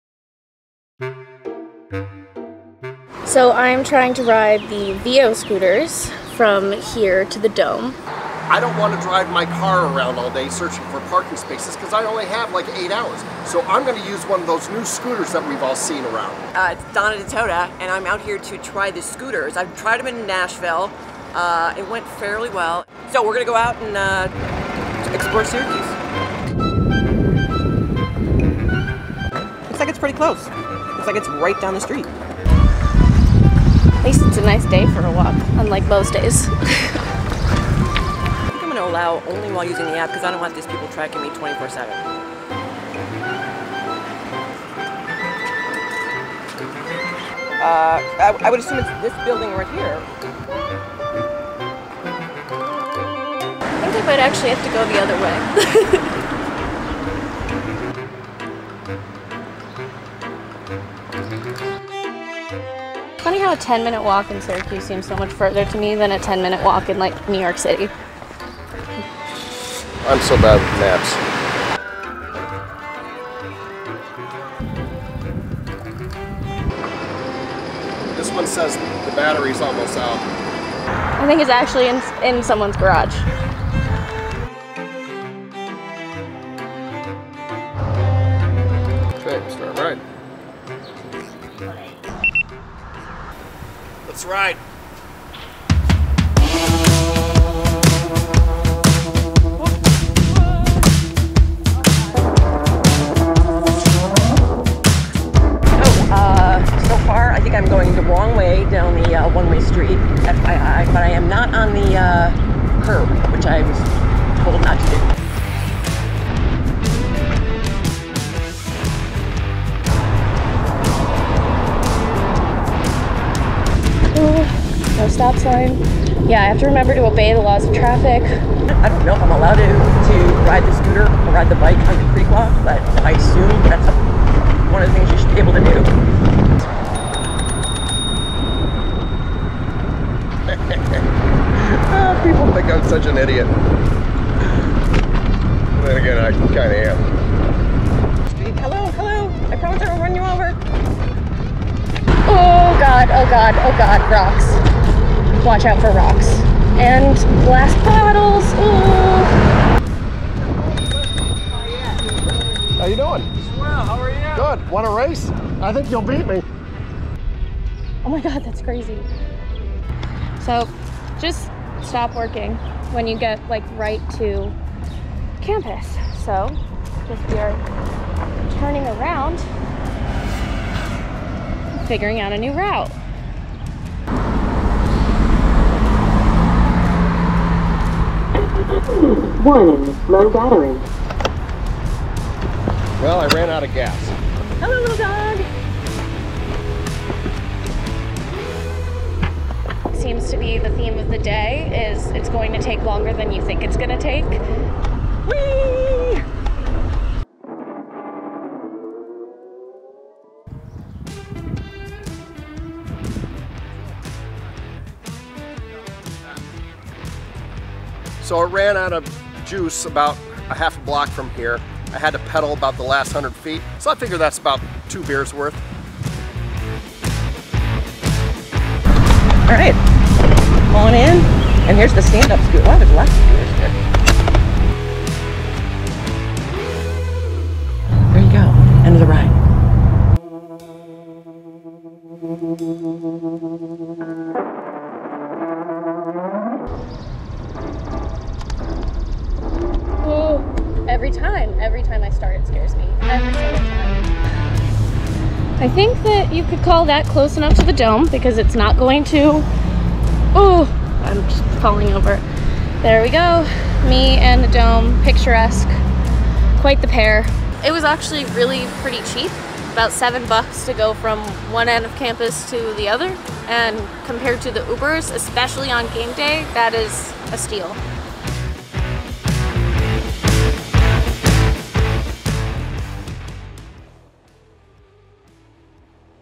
Syracuse.com tested VEO scooters around Syracuse. Are they for you?
Getting around Syracuse has gotten a little easier with VEO scooters available to rent–or has it?
Our reporters Donna Ditota, Anne Hayes, and Charlie Miller check them out going to various parts around the city to see just how easy and reliable the scooters would be so you are better prepared for them.